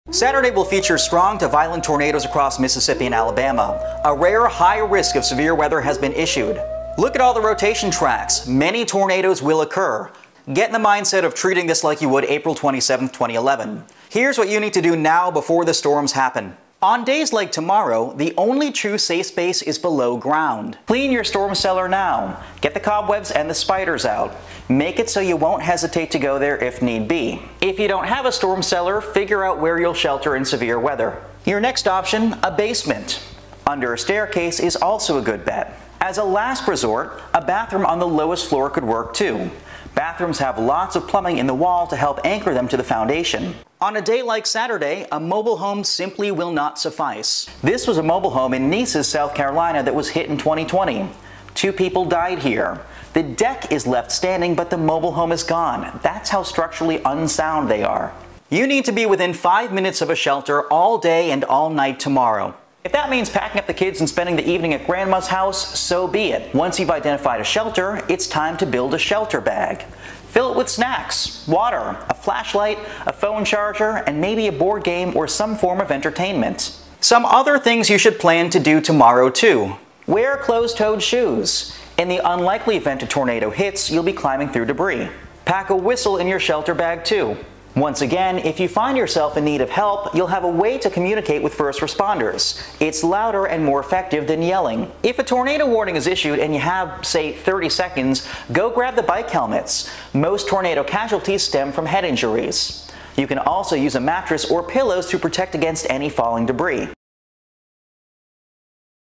HIGH RISK Tornado Outbreak across the Deep South | March 15, 2025
A high-risk tornado outbreak is expected across the Deep South on Saturday, March 15, 2025. Violent, long-track tornadoes, destructive winds, and massive hail are all possible. This setup is eerily similar to April 27, 2011—one of the most devastating tornado outbreaks in U.S. history.